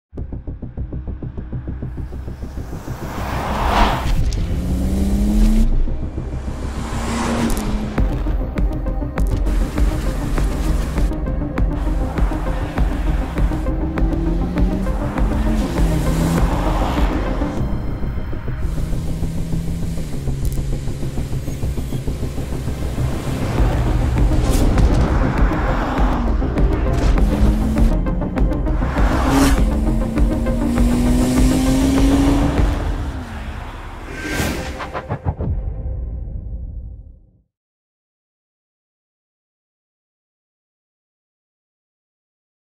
The new Panamera 4S E-Hybrid. Choose boldly.
The new Panamera 4S E-Hybrid is the definition of driving pleasure, offering uncompromising comfort and excellent driving dynamics. It's the perfect blend of a combustion engine and an electric motor that's guaranteed to give you goosebumps.